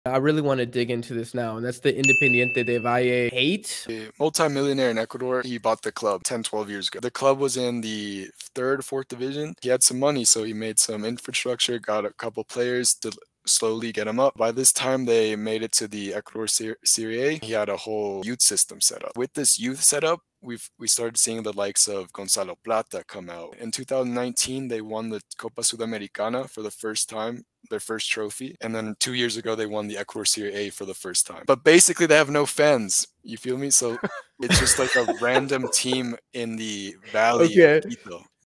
Why Independiente Del Valle Is The MOST HATED CLUB 🤬
The real reason that IDV is the most hated club in Ecuador...